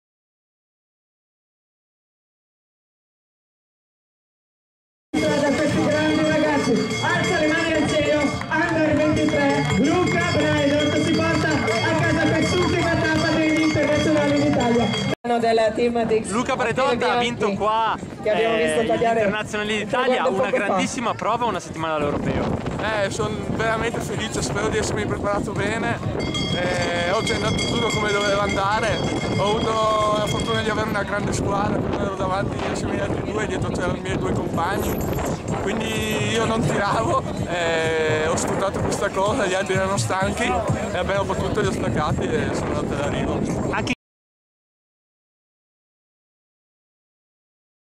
Internazionali d'Italia Vermiglio, vittoria e intervista a Luca Braidot
Vittoria e intervista di Luca Briadot (GS Forestale), primo sul traguardo e vincitore del circuito degli Internazionali d'Italia XC tra gli Under 23